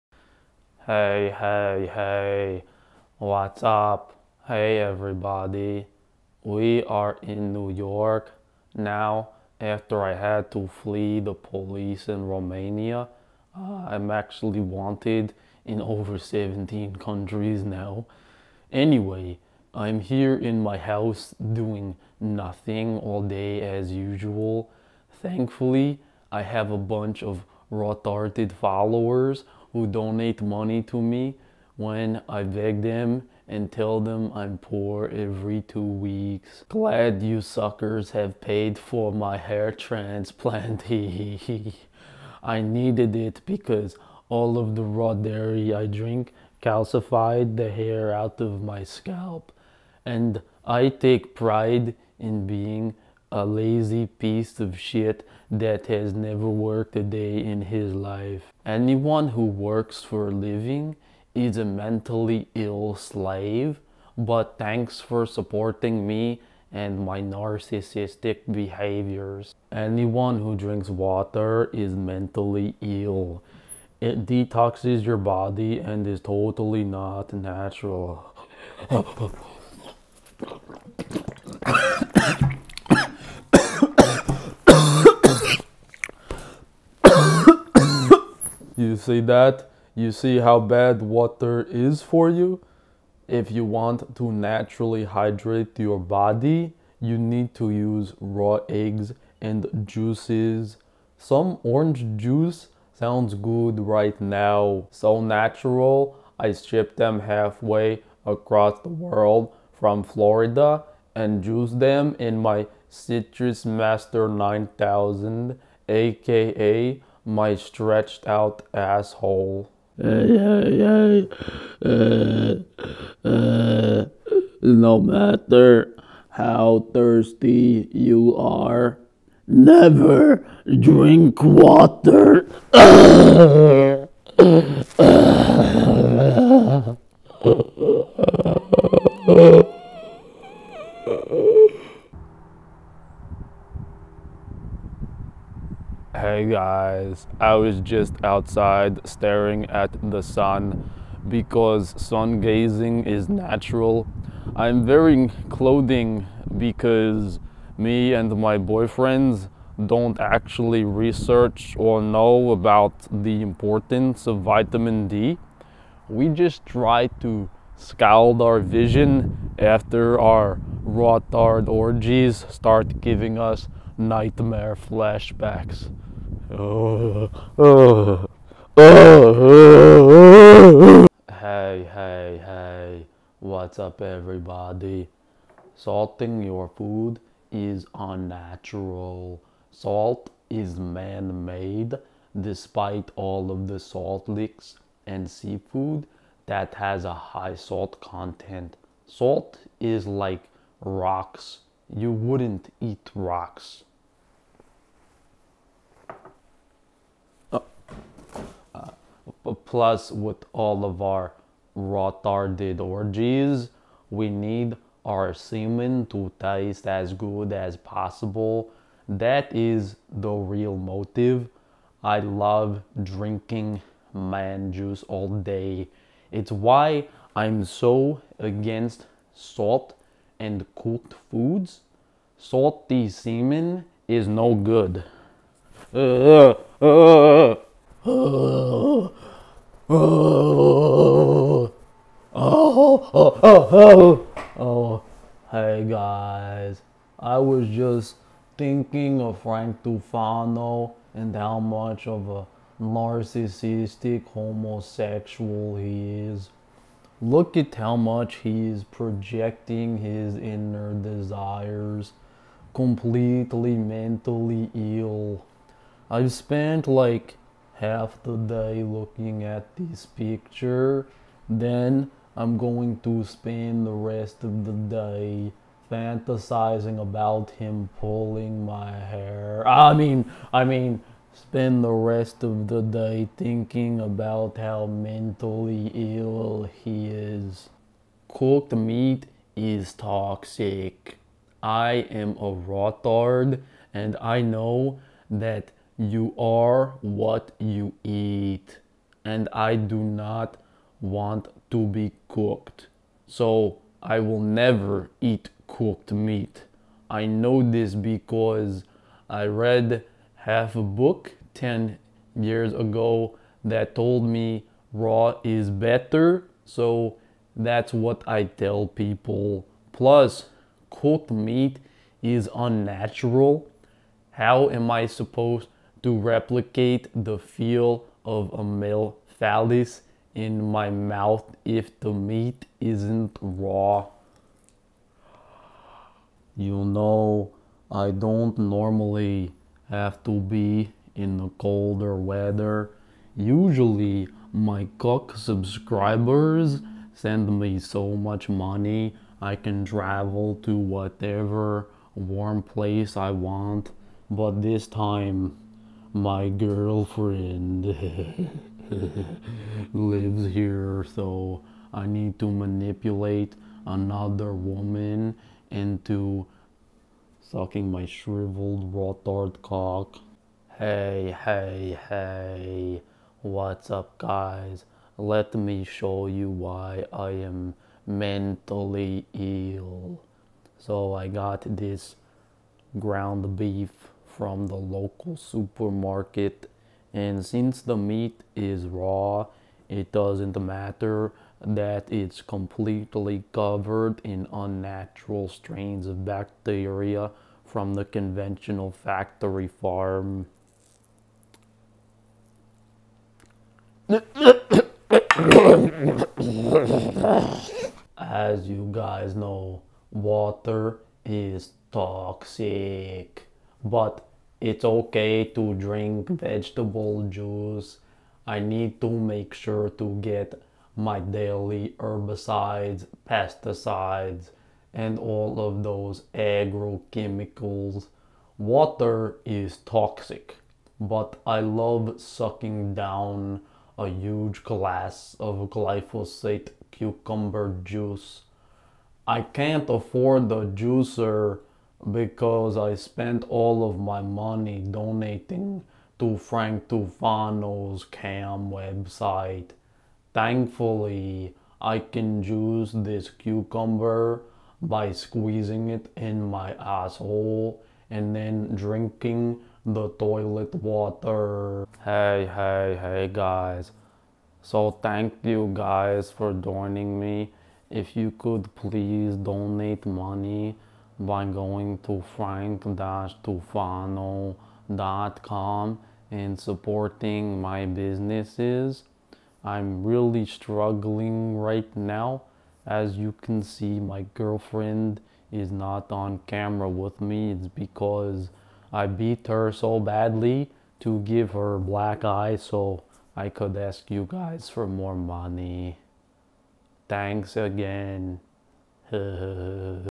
Day in the life of sv3rige
00:00 - Hey hey hey
01:09 - Water is toxic
01:59 - NEVER drink water
02:22 - Sungazing
03:00 - Salt is unnatural
03:57 - Frank Tufano is a narcissist
04:47 - Cooked meat is toxic
05:36 - I am not used to the cold
06:10 - Only fresh raw meat
06:47 - I drink raw juices
07:41 - Thanks for joining